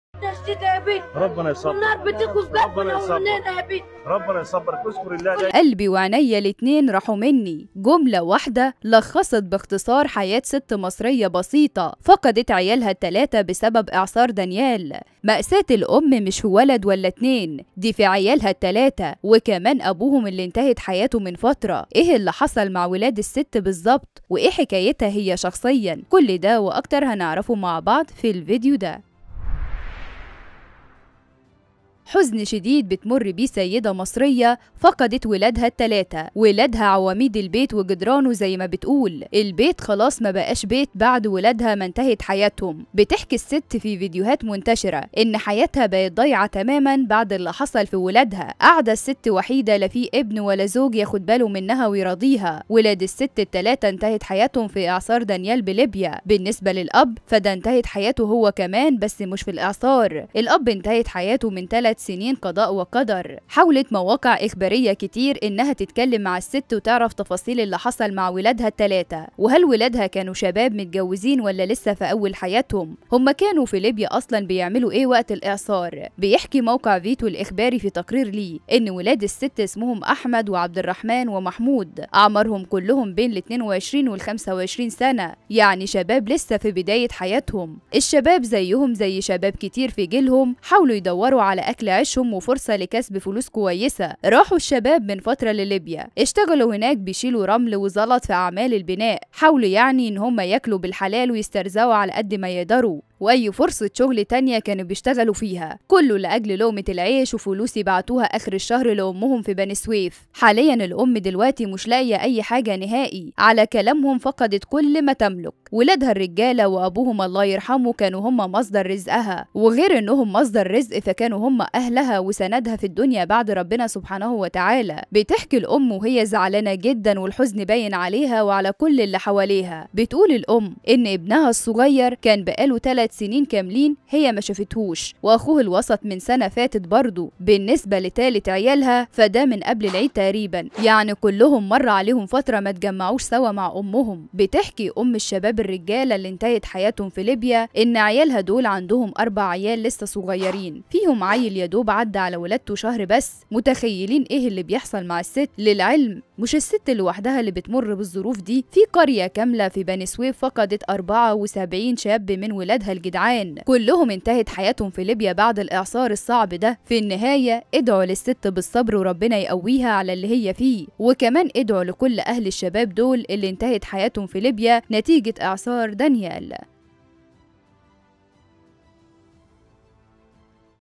أولادها التلاتة انتهت حياتهم بسبب إعصار دانيال.مأساة سيدة مصرية بعد فقدان أولادها الثلاثة ووالدهم!!
حزن شديد بتمر بيه سيدة مصرية فقدت ولادها التلاته، ولادها عماويد البيت وجدرانه زى ما بتقول، البيت خلاص ما بقاش بيت بعد ولادها انتهت حياتهم.فيتو" الإخباري في تقرير ليه أن ولاد الست أسمهم أحمد وعبد الرحمن ومحمود، وأعمارهم كلك بين الـ 22 لـ 25 سنه، يعنى شباب لسه في بدايه حياتهم.